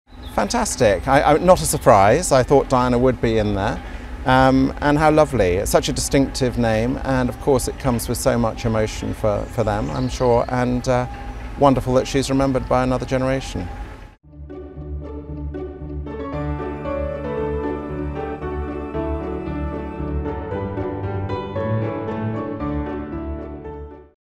Princess Diana's Brother Earl Spencer Hails Harry and Meghan’s Tribute to Princess of Wales
Princess Diana’s brother, Earl Spencer, has said the news of Harry and Meghan’s baby name was “not a surprise” as he thought “Diana would be in there”.

Speaking to ITV News, he also added that it was “wonderful that Diana is being remembered by another generation”.

Report by Roshni Patel.